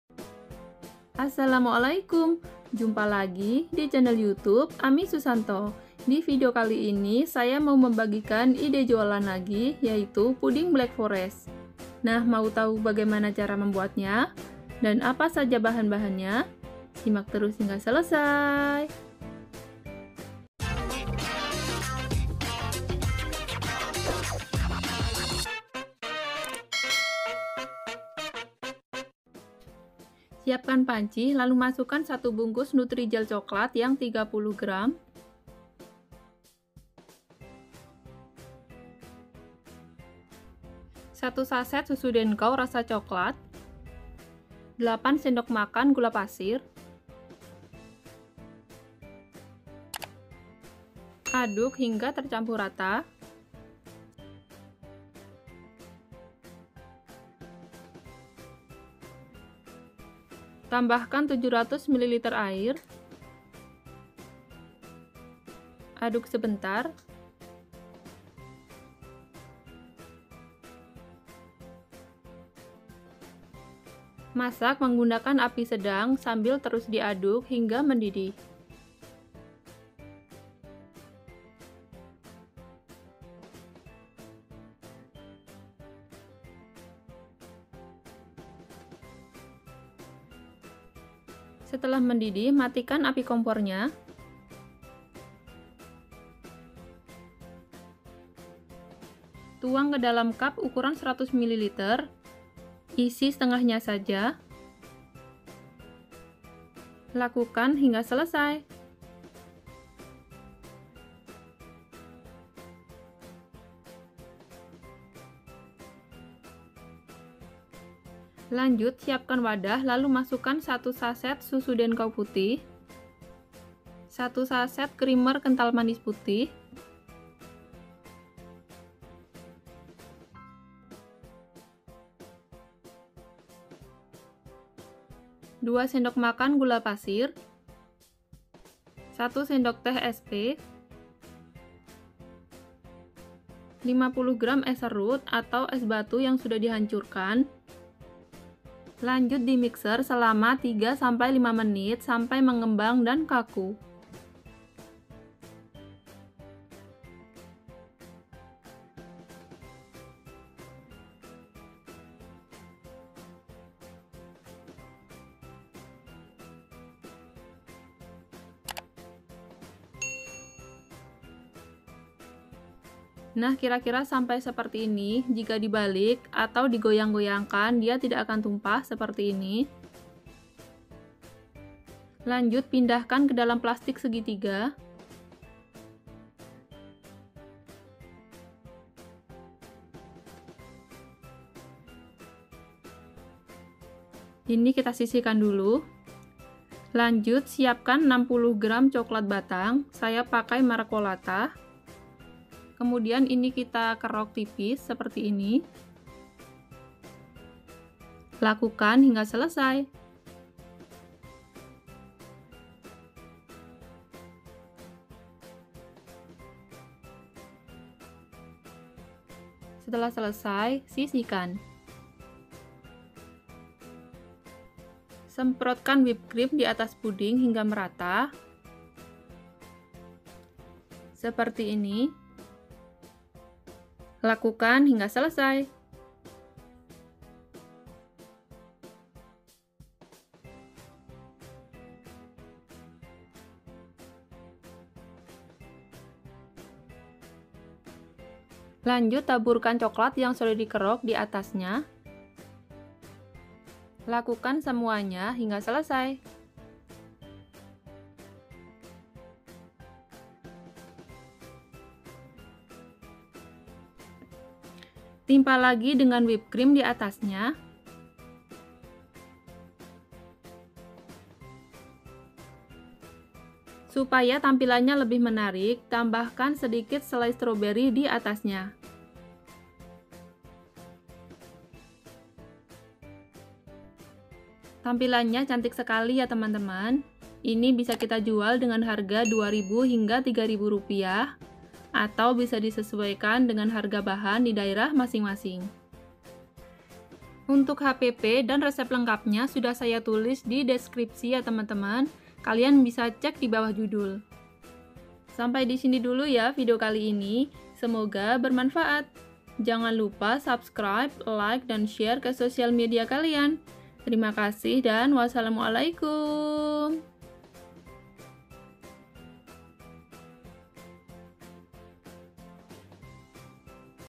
puding blakflores, selalu jadi rebutan dikalangan anak anak maupun orang dewasa
Resep puding blakfores

Bahan puding.
 1 bungkus nutrijell coklat 30 gr - 5500
 1 bungkus susu bubuk dancow coklat - 3500
 8 sdm gula pasir - 1500
 700 ml air - gratis

Bahan wipcream
 1 bungkus susu dancow putih - 3500
 1 bungkus krimer kental manis putih - 1300
 2 sdm gula pasir - 500
 1 sdt sp - 1000
 50 gr es batu gratis

Selai strawberry secukupnya - 1000
Cup kecil - 3000
Coklat batang - 4000

Harga pokol penjualan satu resep
Total = 24 800
15 cup = per cup jatuhnya, Rp 1653
 Buat yang baru mau memulai usaha saya do'akan   smoga laris manis dan sukses selalu